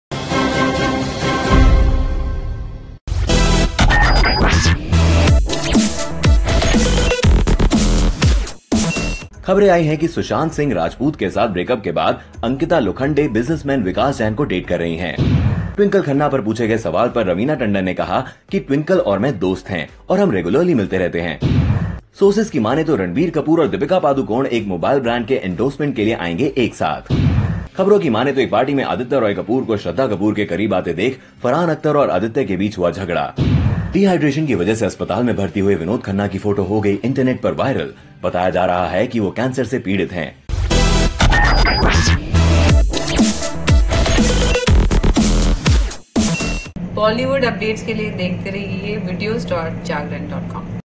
Bollynews Fatafat: We cannot recognize Vinod Khanna in this picture
Is Vinod Khanna suffering from cancer? Is Farhan Akhtar miffed with Aditya Roy Kapur over his proximity with Shraddha Kapoor? For all the breaking news in showbiz, watch Bollynews Fatafat.